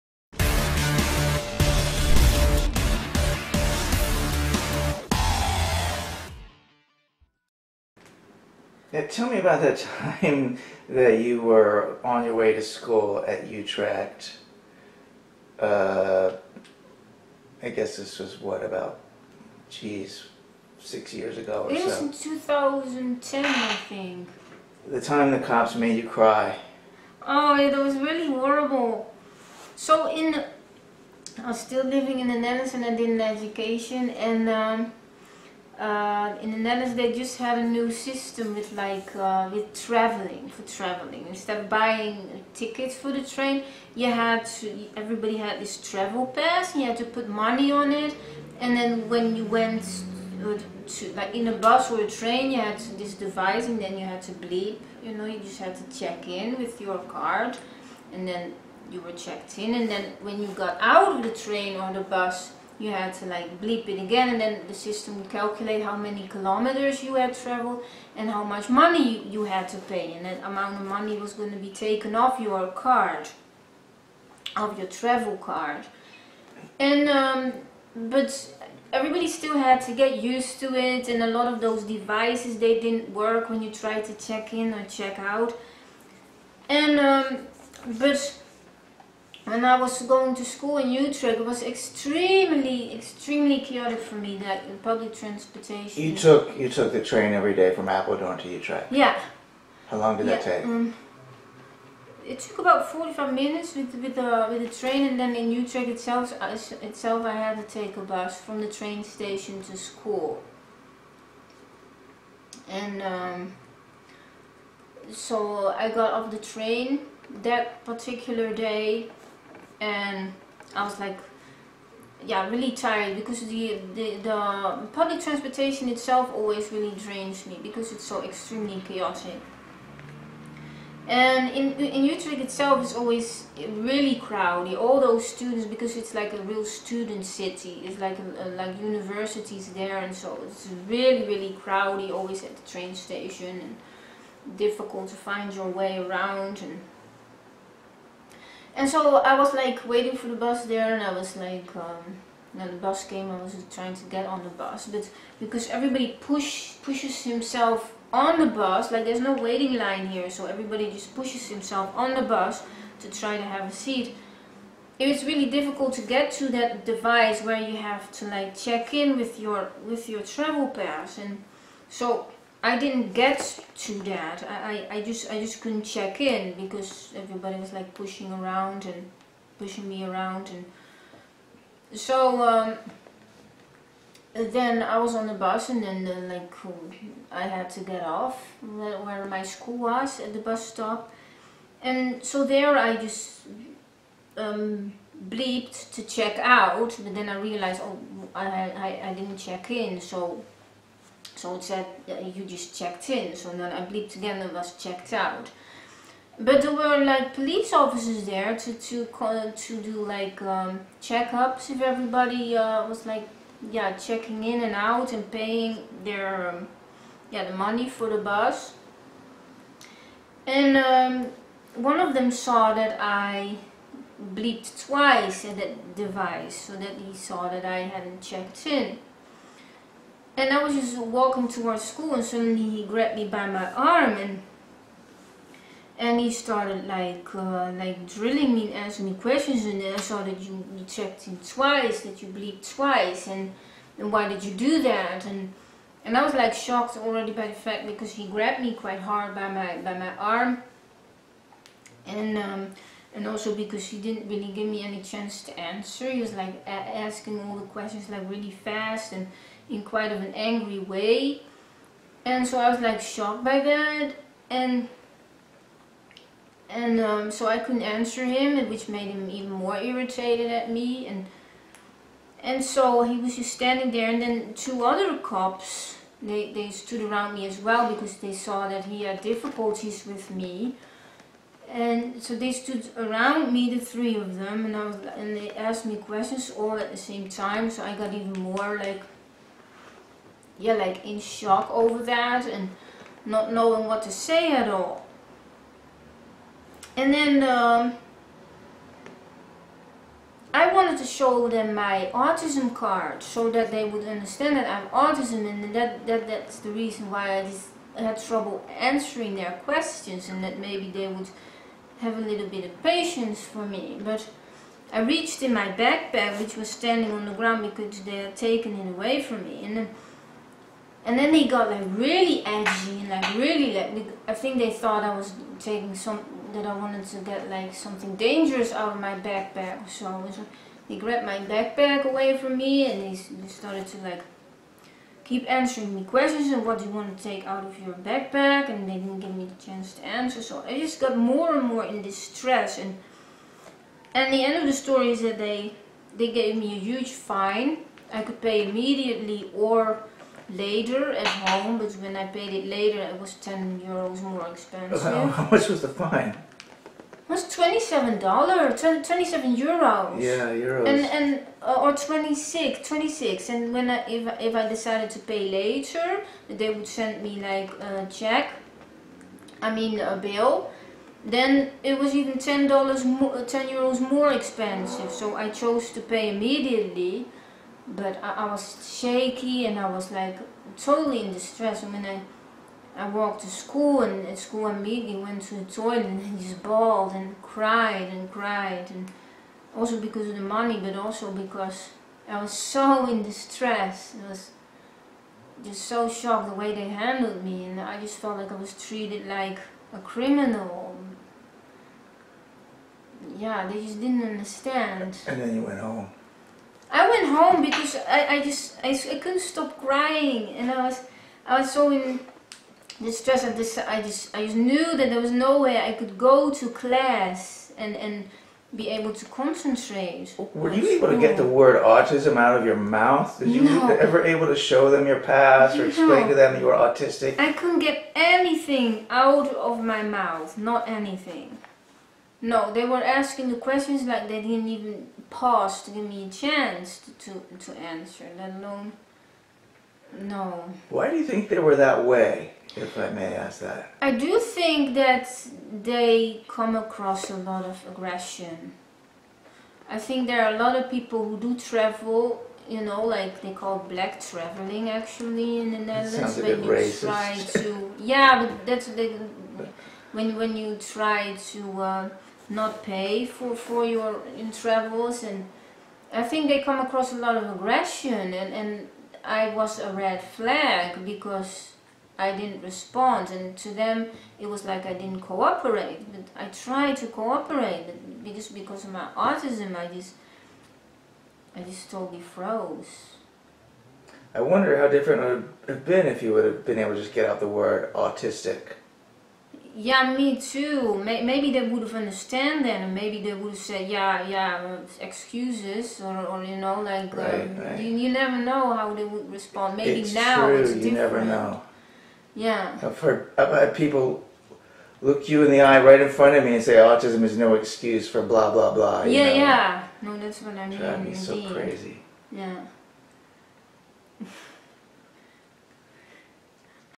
Autistic Artist Talks About Ignorance Part 2 - Remrov's World of Autism #48
Autistic artist Remrov is being interviewed about ignorance towards autism. In this episode he shares an experience that happened during public transportation with transit cops.

If you like to see Remrov's drawings, have a look at his